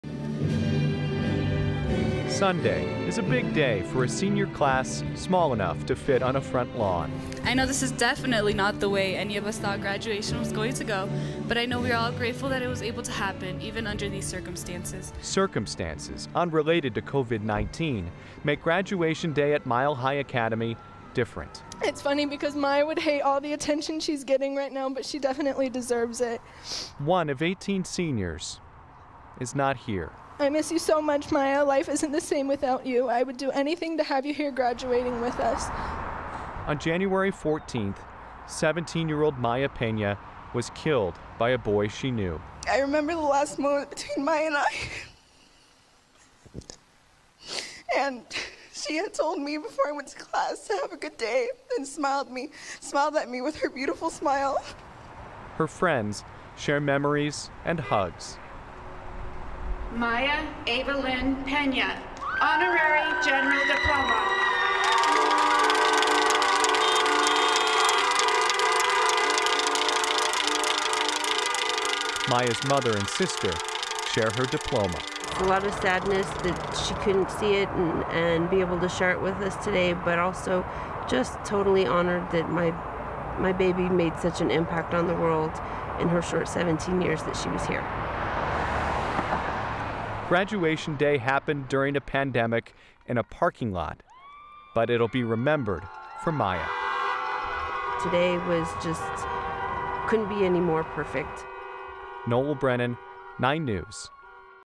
Mya Pena would have graduated today – the 17-year-old was killed in a murder-suicide in Douglas Coun
Seniors honored their friend and classmate Sunday morning at Mile High Academy in Highlands Ranch.

9NEWS (KUSA) is located in Denver, Colorado.